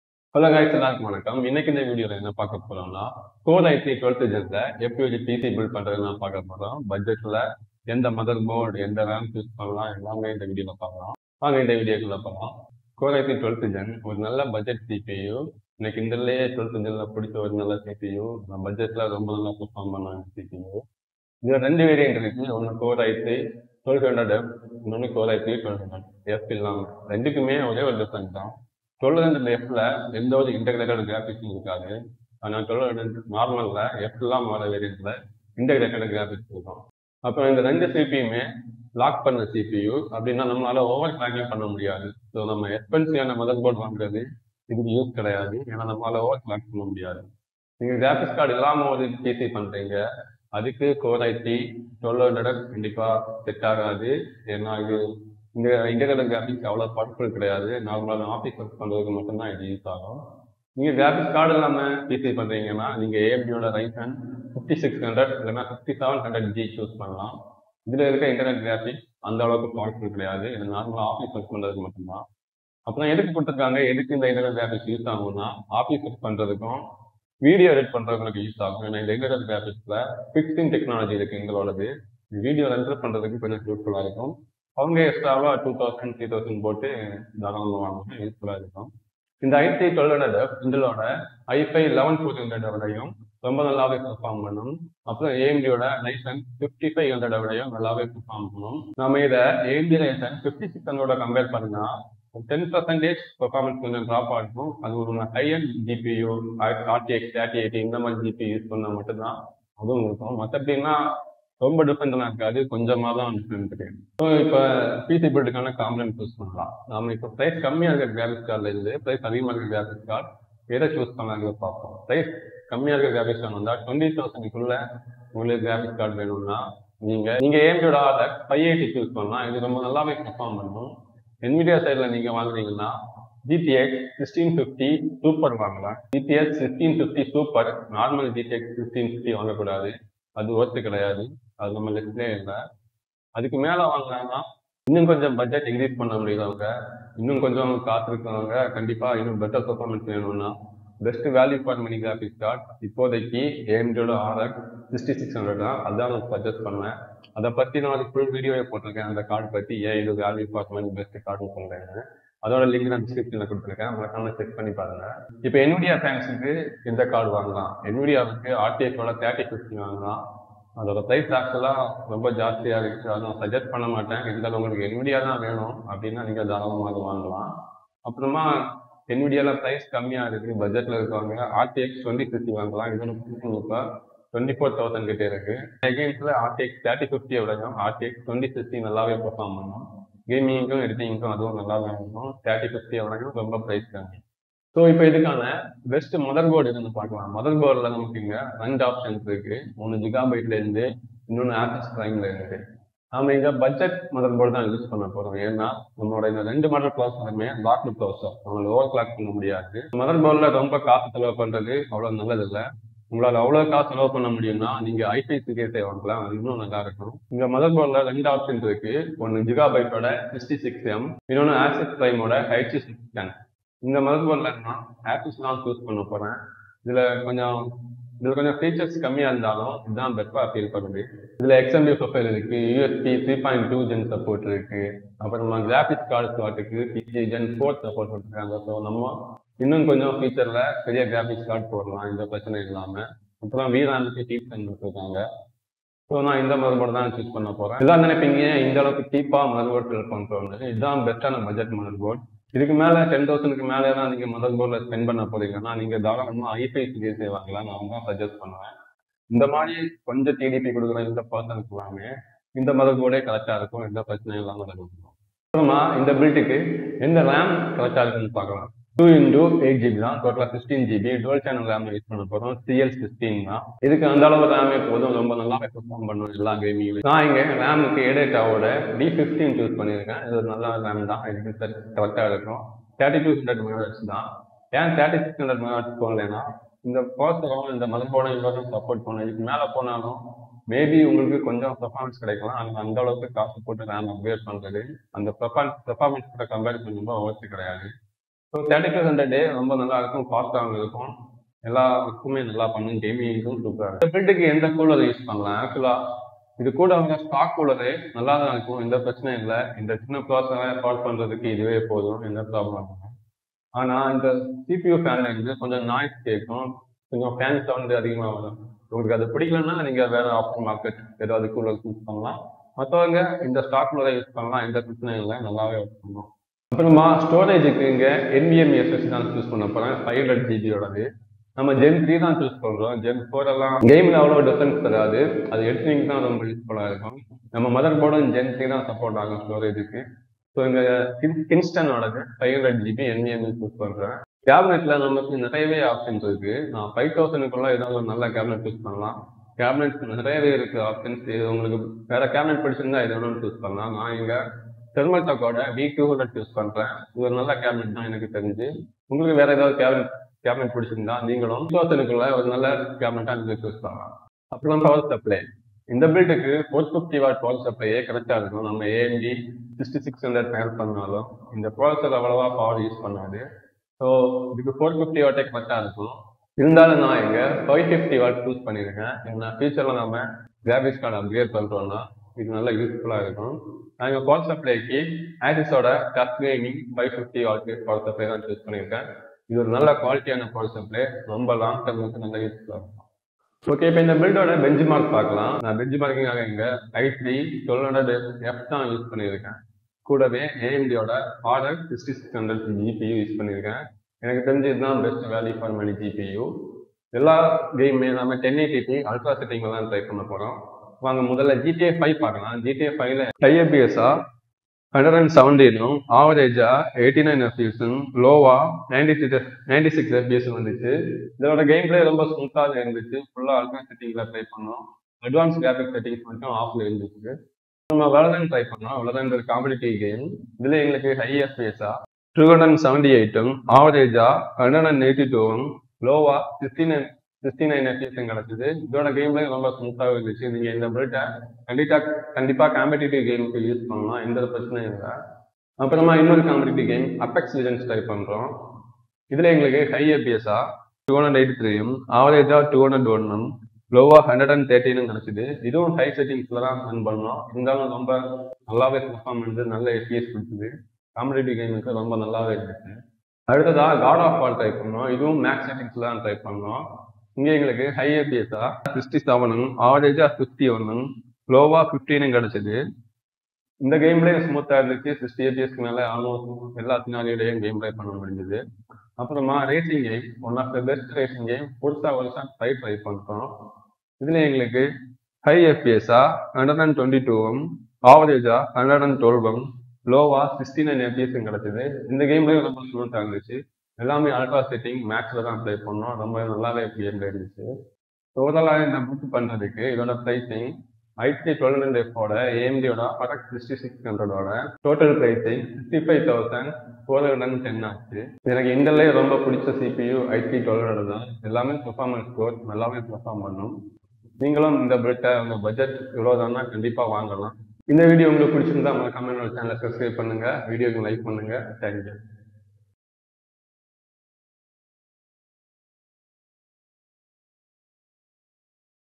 2022 Best intel i3 12100F Gaming pc build with best budget parts Tamil
I've created this new pc build for people trying to find a reasonable priced intel i3 12100F gaming pc build.

0:00 intro
0:22 Cpu
1:29 cpu Comparison
2:03 budget gpu
2:32 best gpu for i3
3:39 motherboard
4:26 best ram
5:04 best cooler
5:28 storage
5:44 cabinet
6:05 psu
6:33 Game Benchmark
8:18 Build price
8:38 support